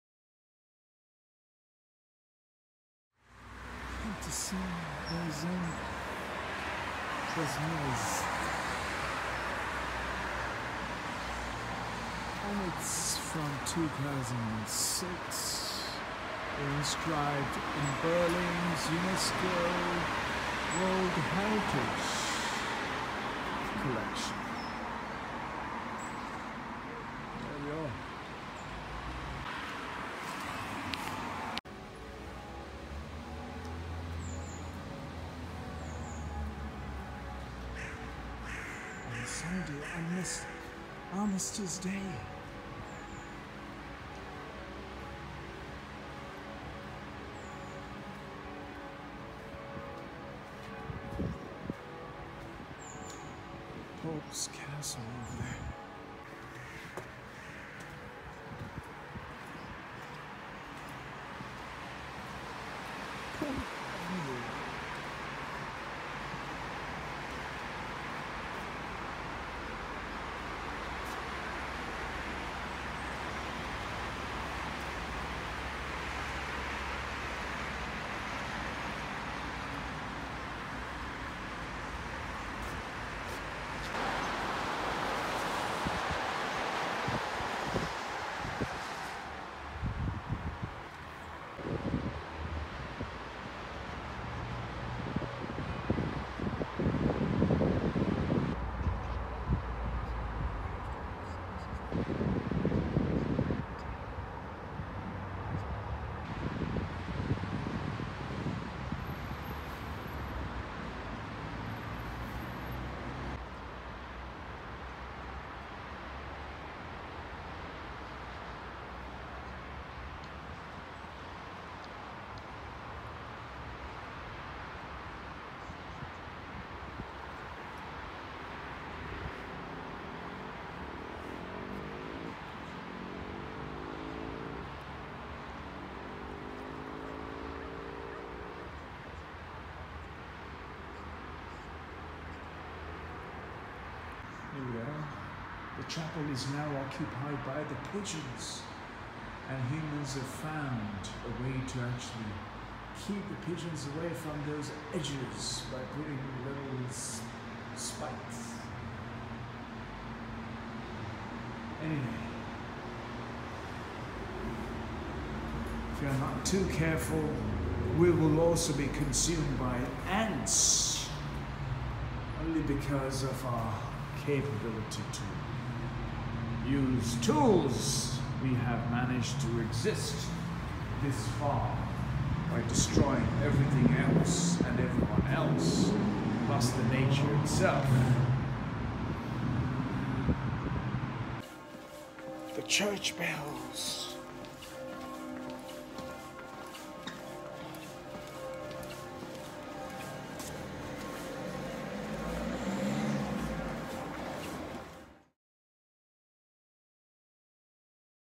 Pont Saint Beneset Avignon France UNESCO
Pont Saint-Bénézet also known as the Pont d'Avignon is a famous medieval bridge in the ... It was destroyed during the siege of Avignon by Louis VIII of France in 1226 but beginning in 1234